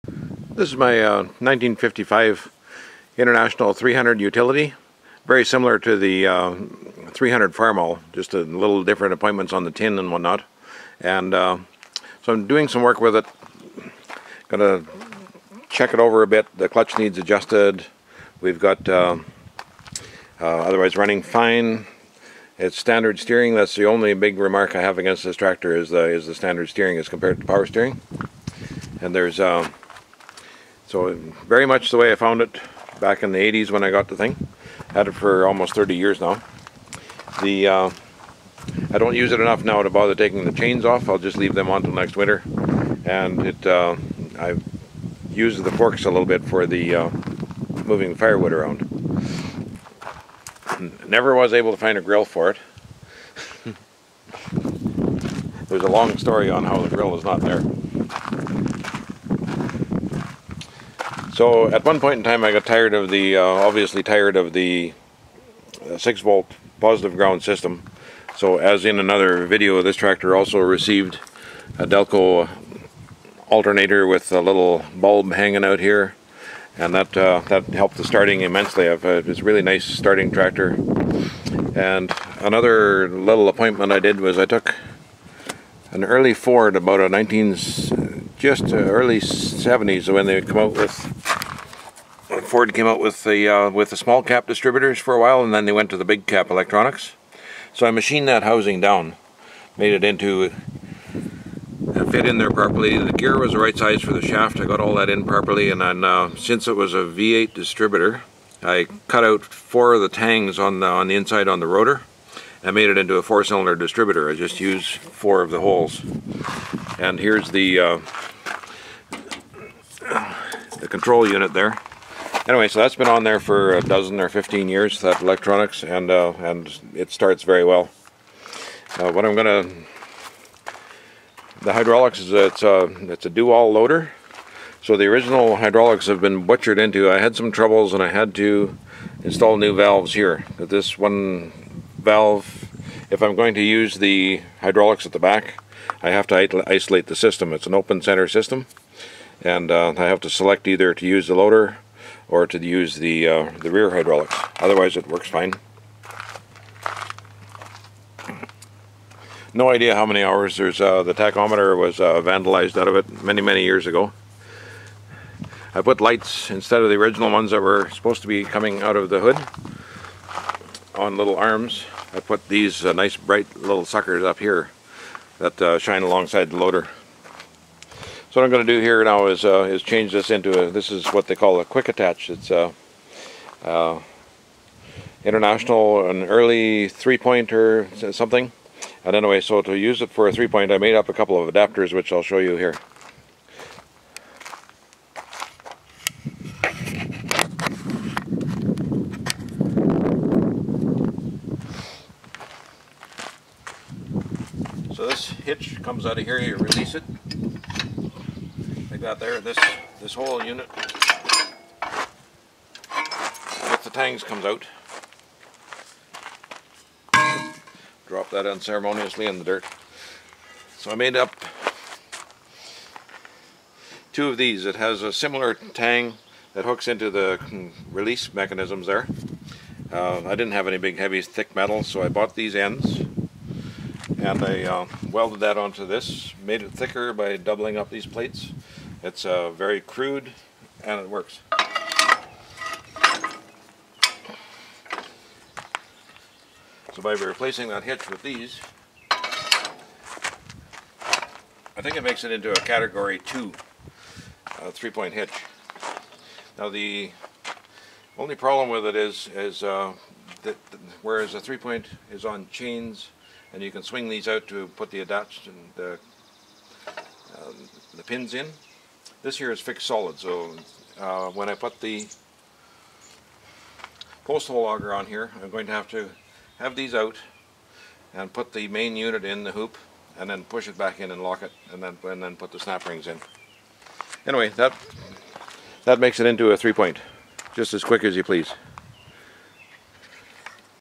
IHC/Farmall - DIY elect. Ign. - Fast-Hitch changed to 3 Point Hitch - zeketheantiquefreak
IHC/Farmall - Late '70's? Ford electronic ignition adapted to IHC 300 Utility. - Fast Hitch changed to 3 point hitch